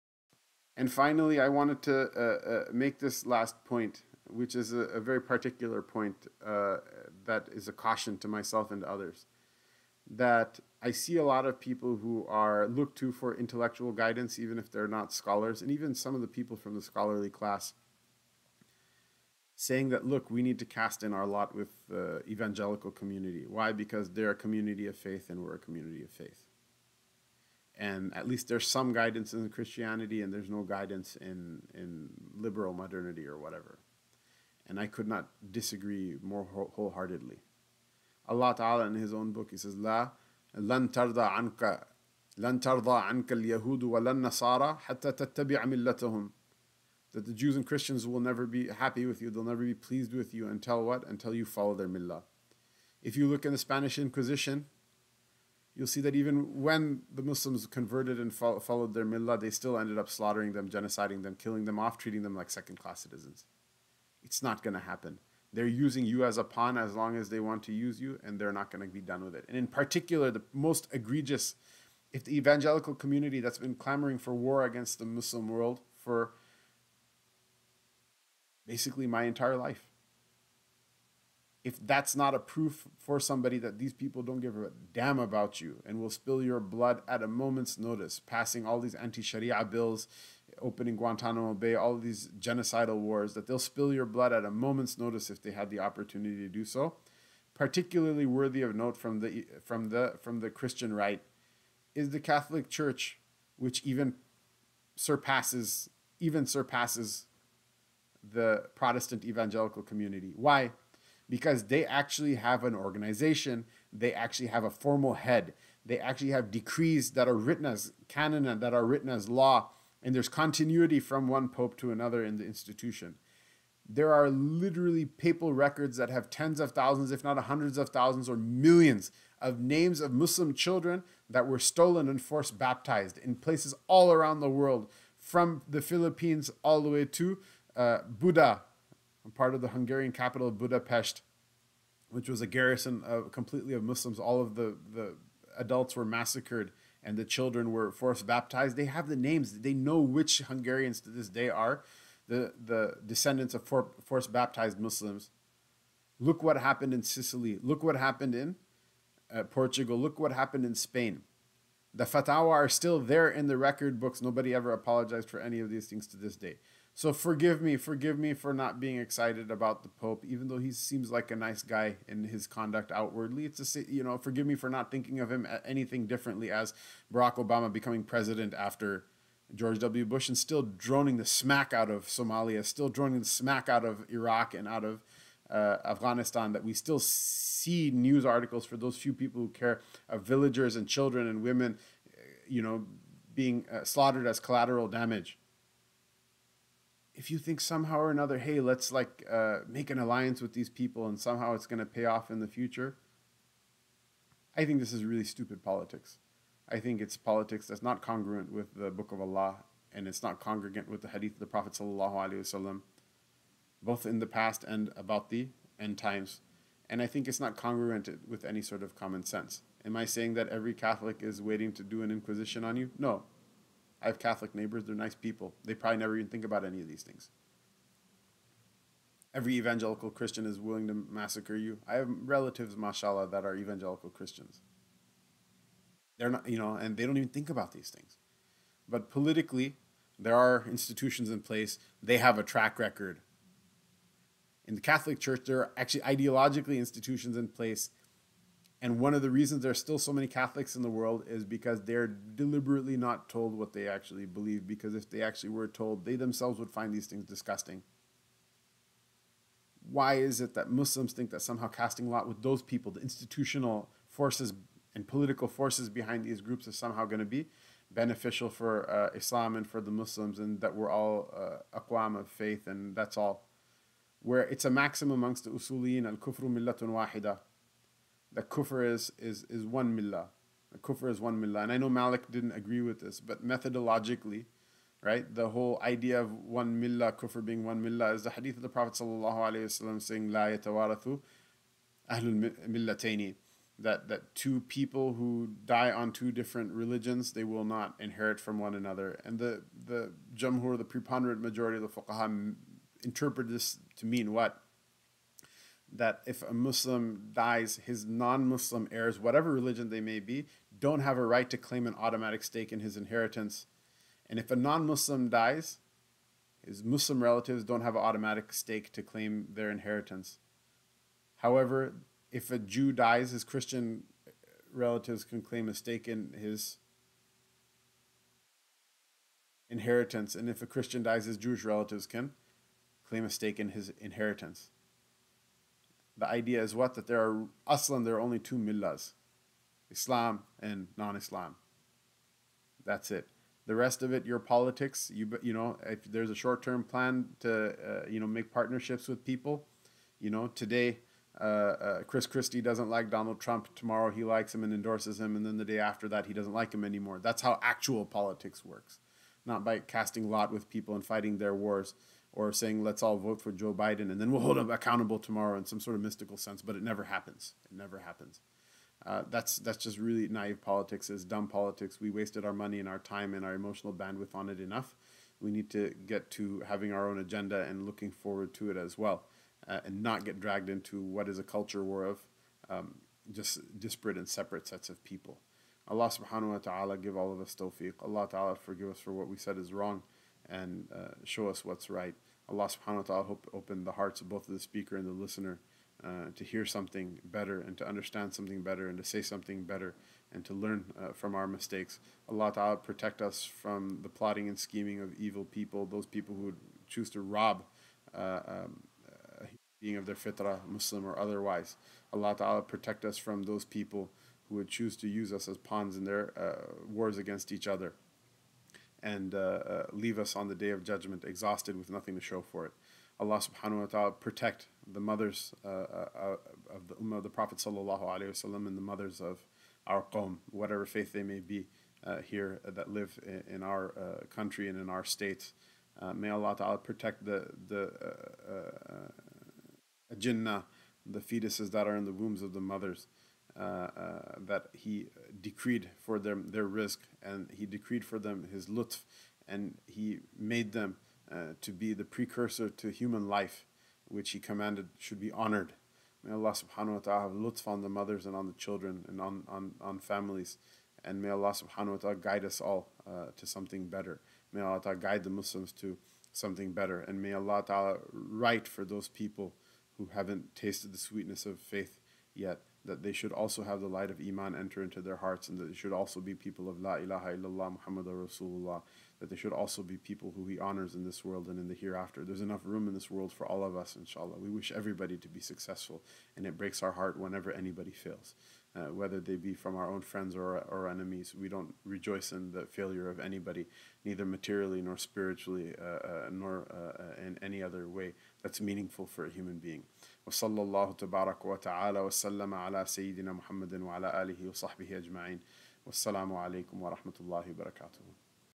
The Folly of Preferring and Casting Lot in With One Error Over Another
Allah, Most High, says in His book: "The Jews and Christians shall not be pleased with you until you follow their way."

It is a very established creedal maxim that all of disbelief is one community.

Then why is it that many prominent voices within the Muslim community seem to think that some sort of Grand Alliance of Faith is possible between those who advocated, consented to, and harbor desires for genocide of entire nations of the earth, mostly Muslims, in front of their very eyes?

It is extremely naive and not good politics.

I'm not saying that all evangelicals or Catholics are like that as individuals, but politically, institutionally, and in the case of the latter, canonically, they have a very antagonistic track record. To ally on ad hoc issues makes sense. To think that we have a greater common goal, is just delusion.